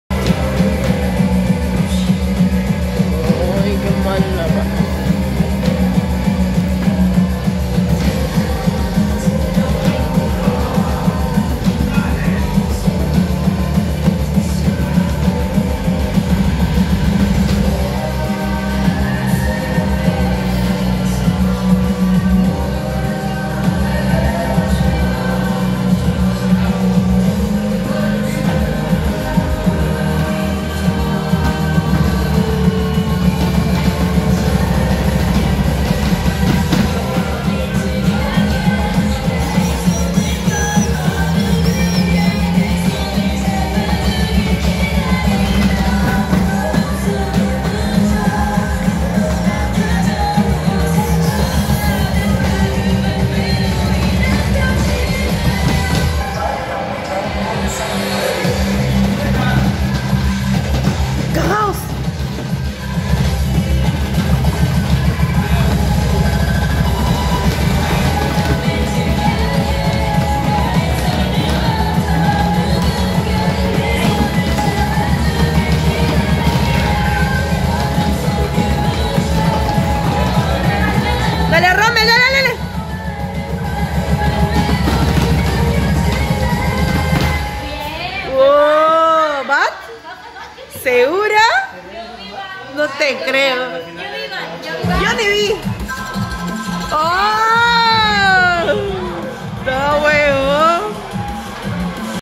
Alexis vs. Romel: Cross Soul S22*
Un día de versus! Entre Alexis "la Rata" y Romel Montoya :D

Fueron unos versus de Infarto!!! Aquí les dejo algunos ;D